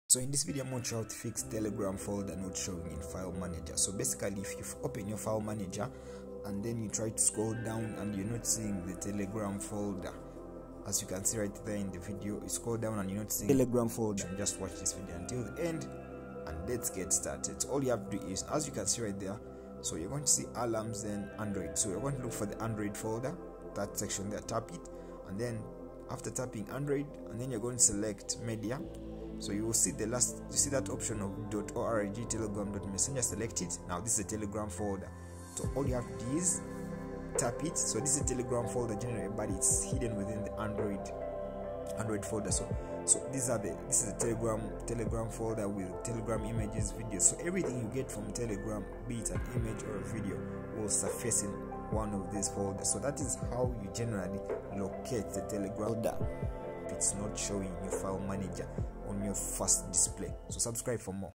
how to fix telegram folder not showing in file manager Android 2022
in this video I am going to show you how to fix telegram folder not showing in file manager Android